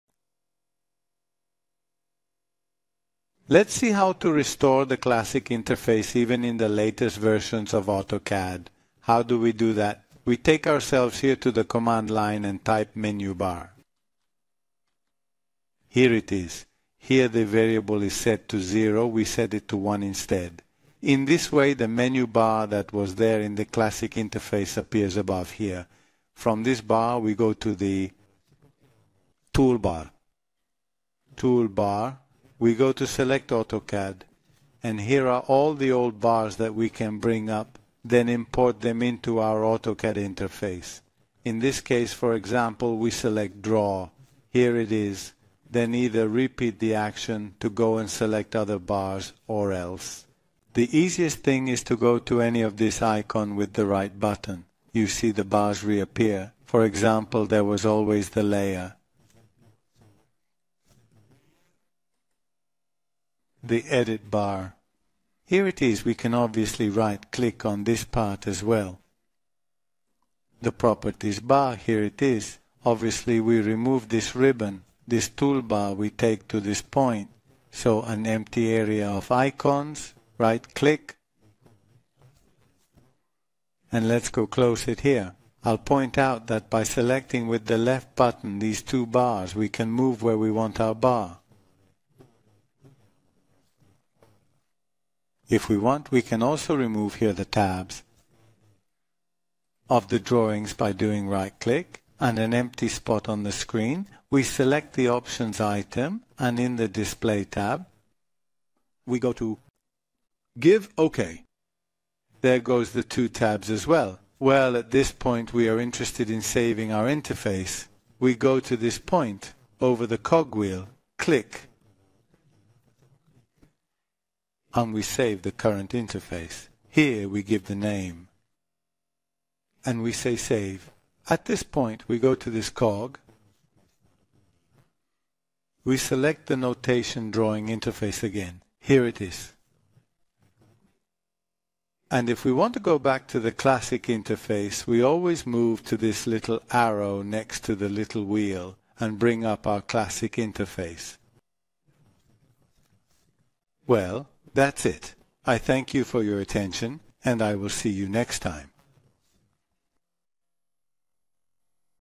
How to restore the CLASSIC INTERFACE in AutoCAD
Just a few easy steps, we restore the dear old CLASSIC AutoCAD INTERFACE.